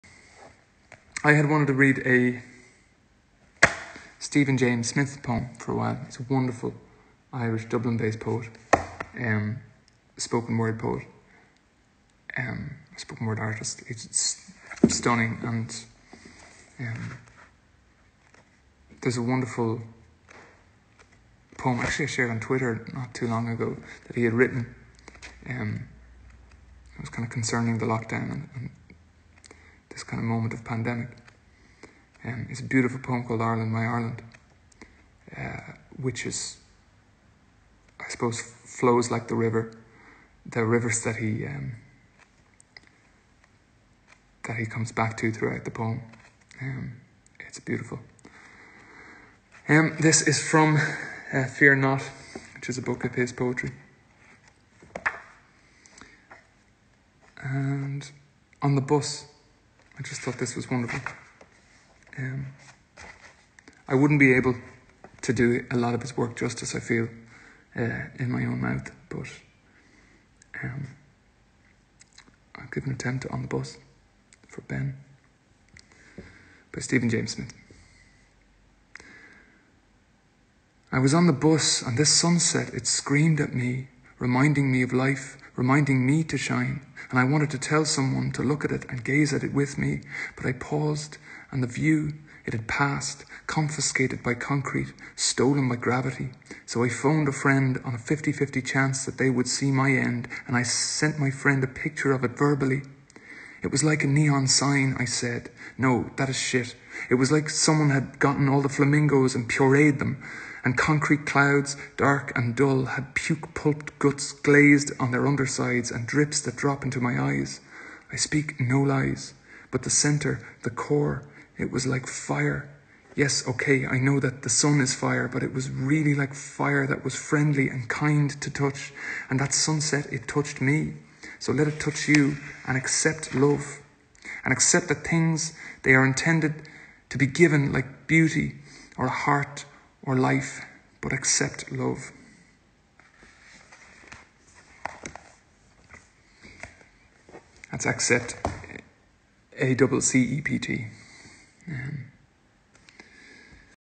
Hozier - IG Live - 17/07/2020 - 'On The Bus' by Stephen James Smith
Huge thanks to Hozier for reading my poem ‘On The Bus’ during his Instagram live streaming on the 17/07/2020